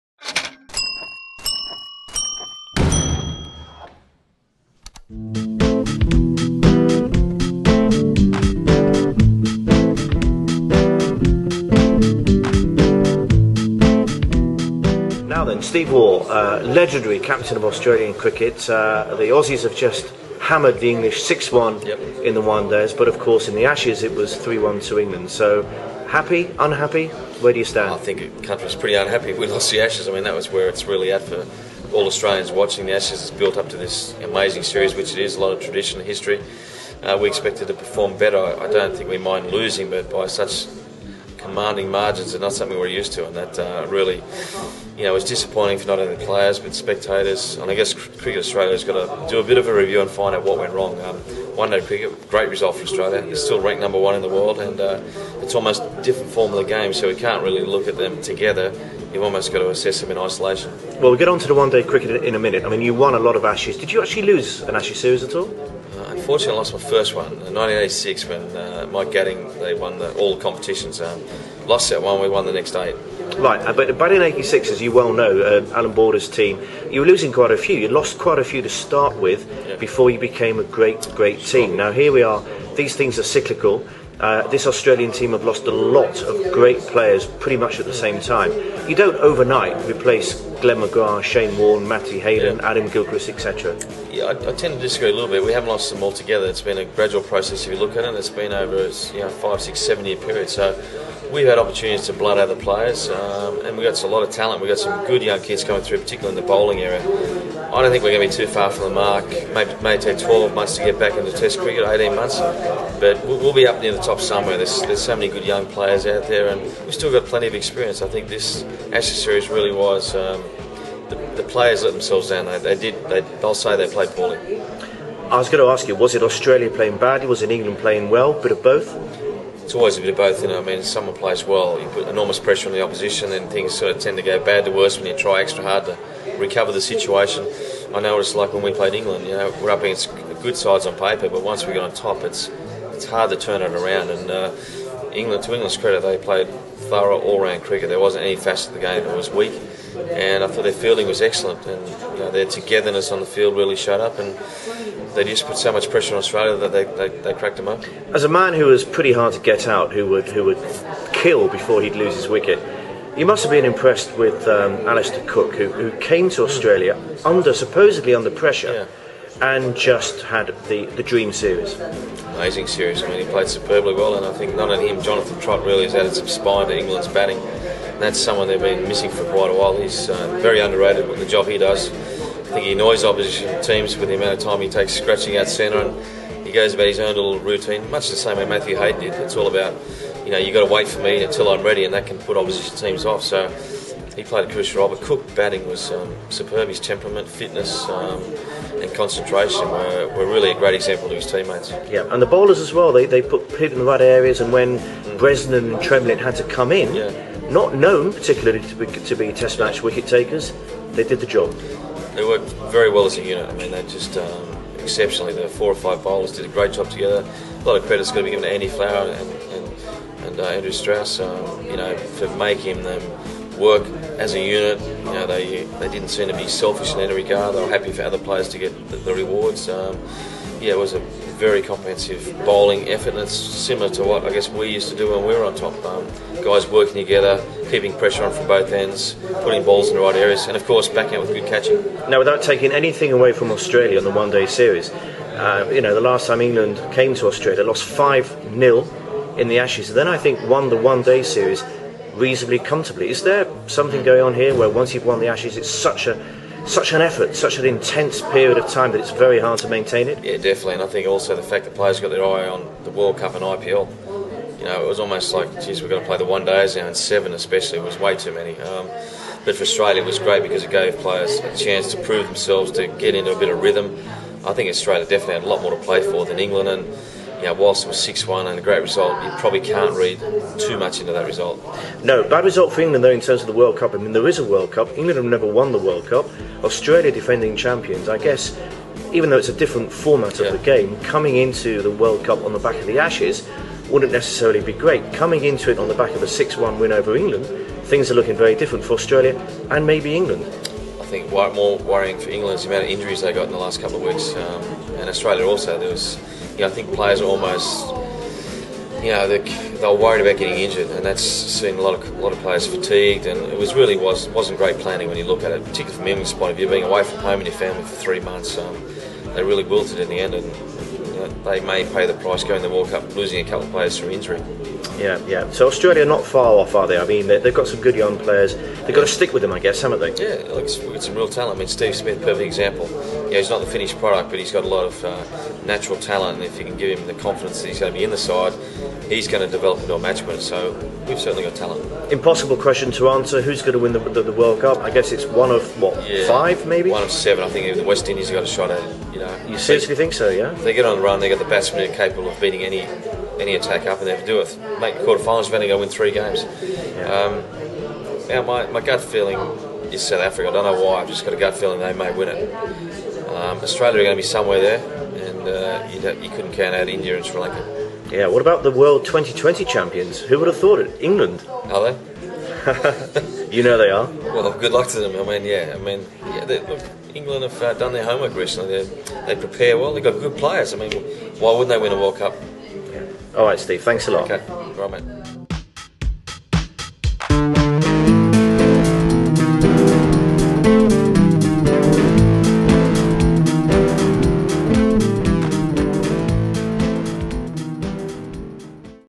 Steve Waugh - Sportsvibe TV
Sportsvibe grabbed a word with legendary Aussie skipper Steve Waugh in Abu Dhabi.

See Steve contrast England's Ashes victory with One-day international defeat, talk about the fall and rise of Australian test cricket and preview the Cricket World Cup.